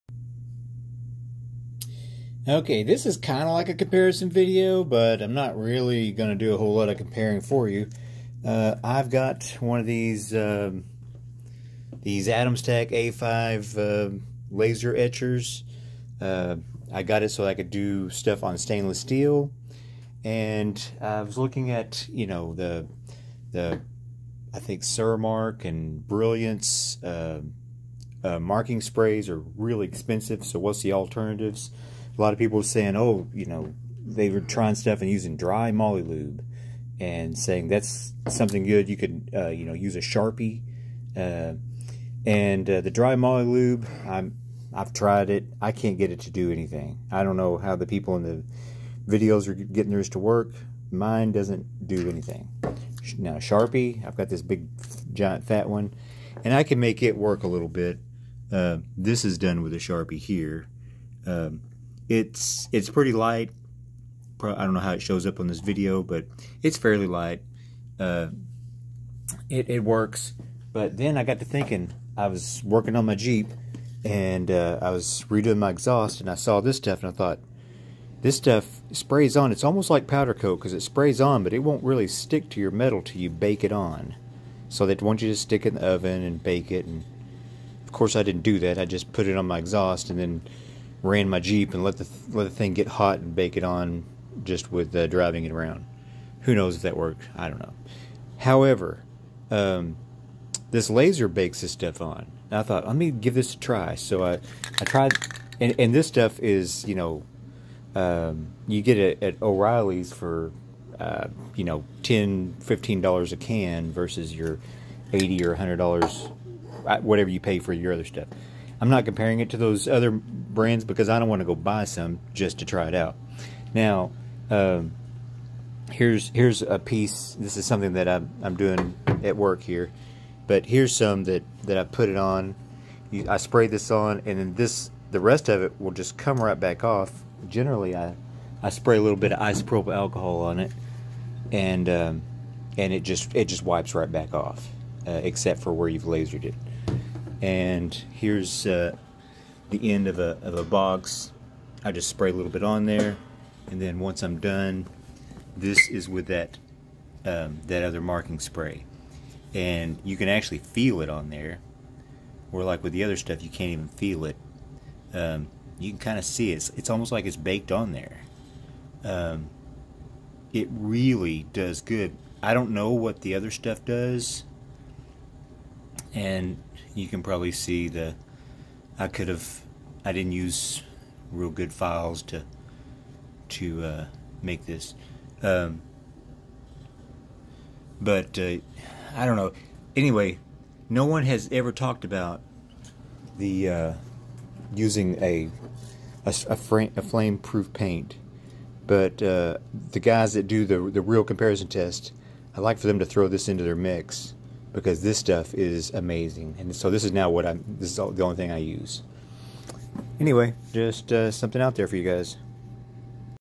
laser etching Stainless Steel alternative marking spray.
I was looking for an alternative to the high-dollar marking sprays to etch my stainless steel parts.  I watched other comparison videos and tried what they were doing,  but couldn't get the results that I wanted.   then I had a thought and tried this exhaust spray.  I was so amazed at the outcome.  I am also wondering if I tried any of the color exhaust sprays if they will go on in color.  I don't know yet.  If anyone tries it,  let me know.

Also, I am just using the freeware LaserGBL and my settings are just what it gives me for engraving SS. I haven't tweaked it any because it was perfect on the first run and I don't have time for that.